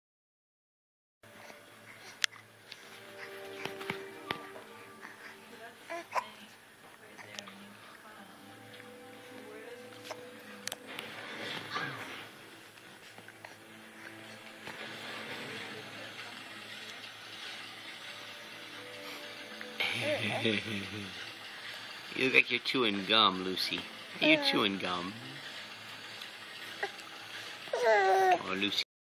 funny faces
The twins making some seriously funny faces